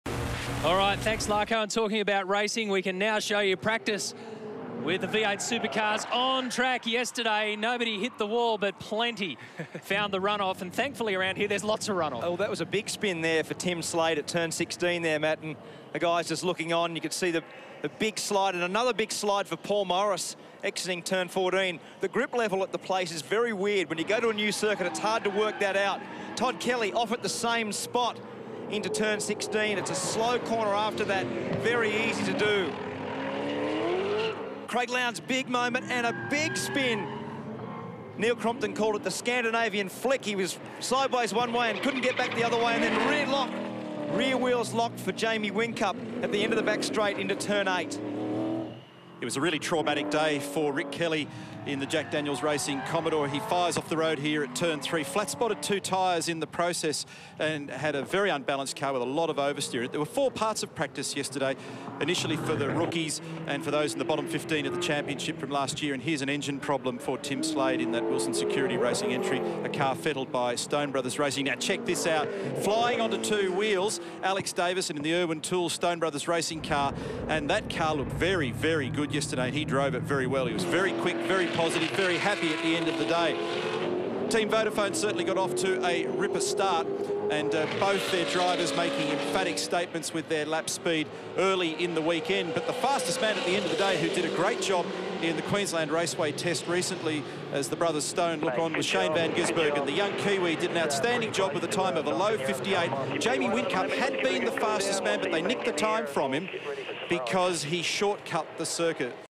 V8 Supercars : Practice Highlights (Abu Dhabi 2010)
Highlights of the practice session in Abu Dhabi